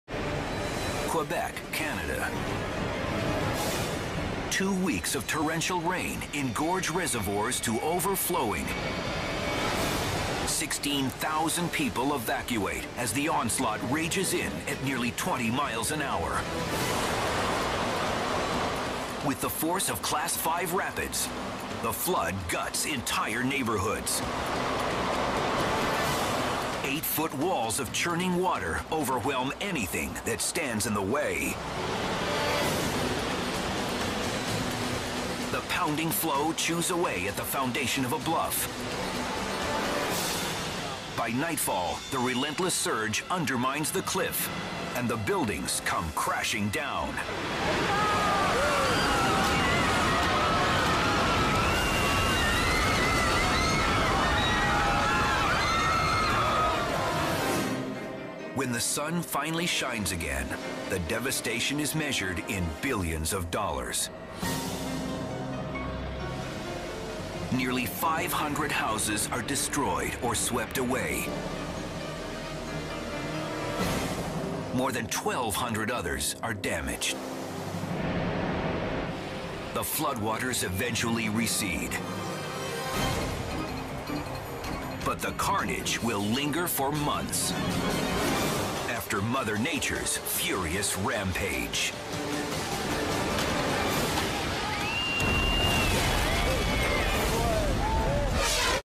Saguenay Flood Of 1996
The Saguenay flood (French: Déluge du Saguenay) was a series of flash floods on July 19 and 20, 1996 that hit the Saguenay–Lac-Saint-Jean region of Quebec, Canada. It was the biggest overland flood in 20th-century Canadian history.[1]